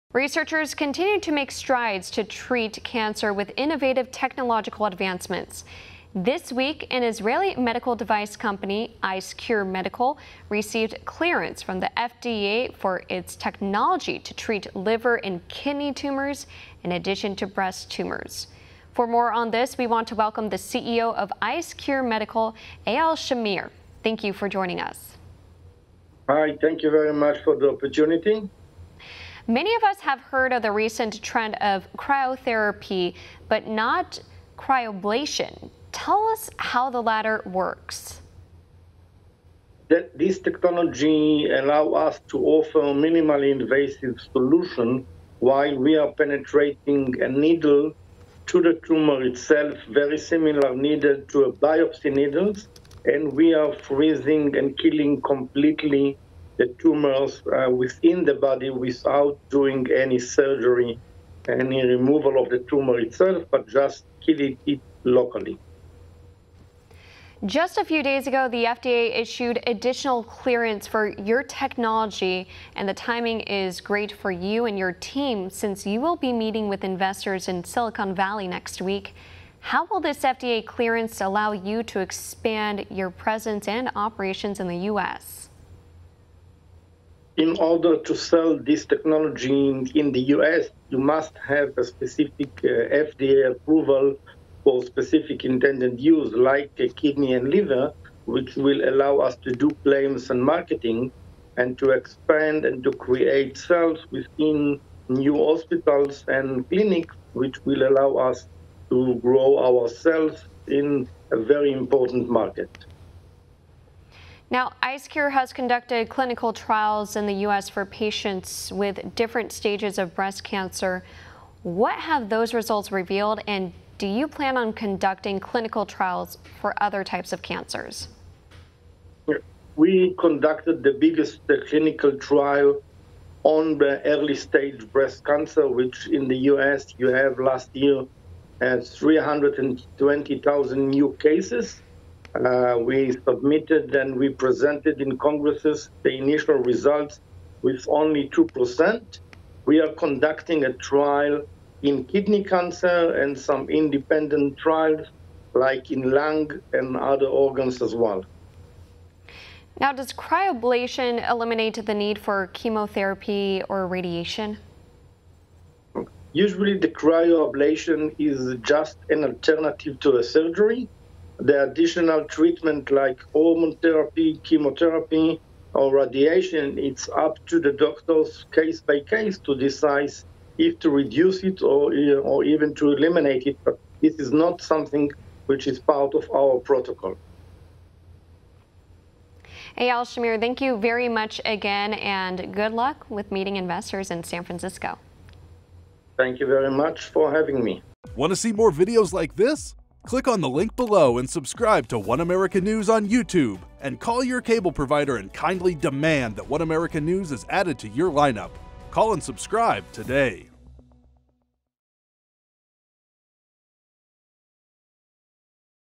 Groundbreaking tumor treatment technology receives FDA clearance
IceCure on One America News